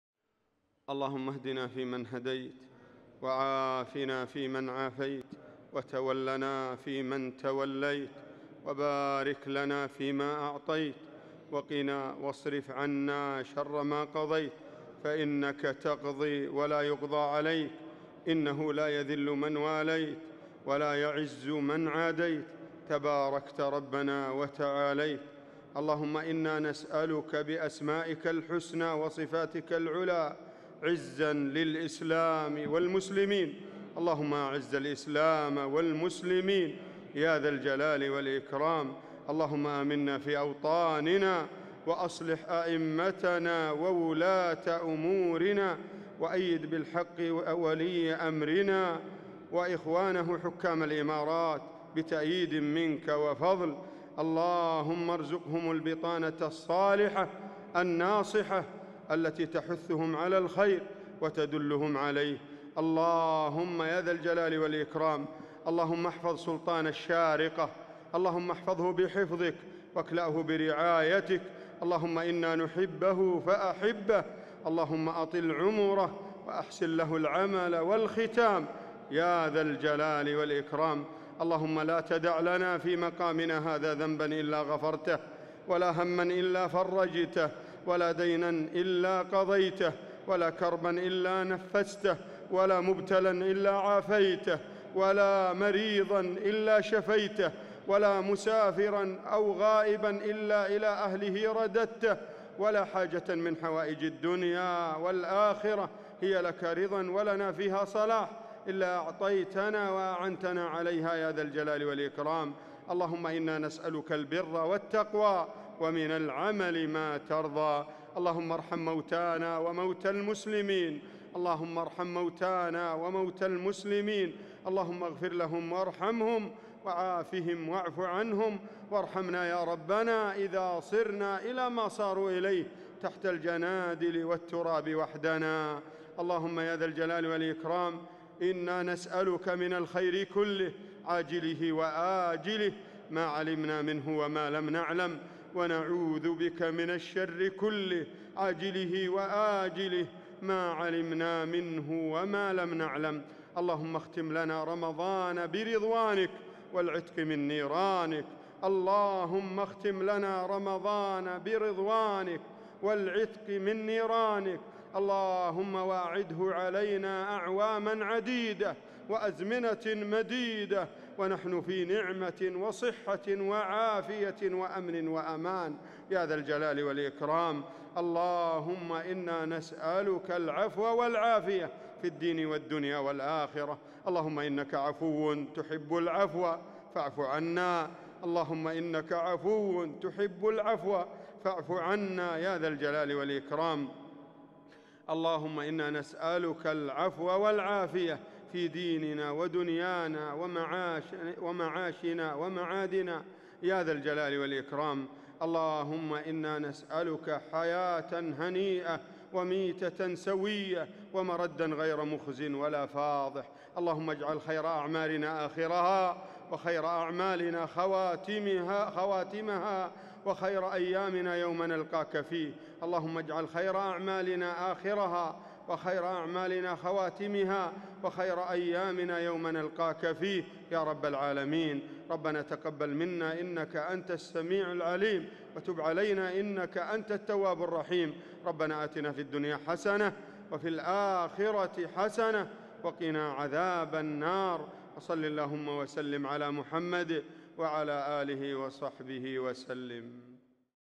دعاء القارئ عوض حسن حشوان | ليلة 29 رمضان 1440 | مسجد القصباء
فعالية قراء الشارقة في نقل صلاة التراويح من مساجد الشارقة 2019 | 1440 هـ